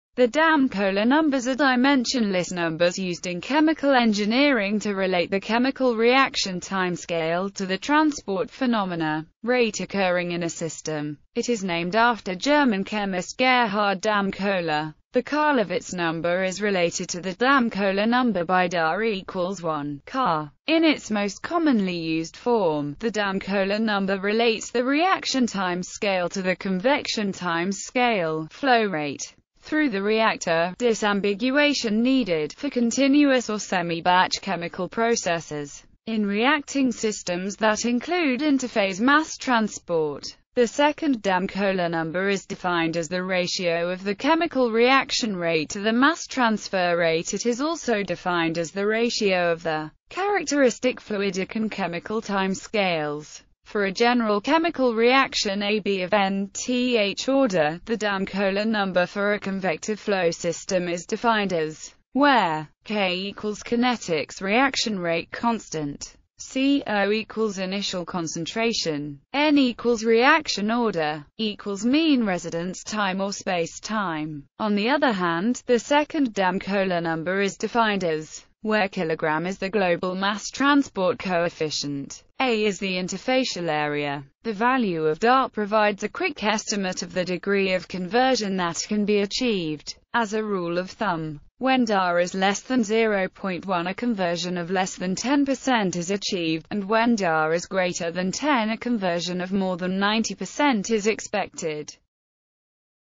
Damköhler numbers
The Damköhler numbers (Da) are dimensionless numbers used in chemical engineering to relate the chemical reaction timescale (reaction rate) to the transport phenomena rate occurring in a system.It is named after German chemist Gerhard Damköhler.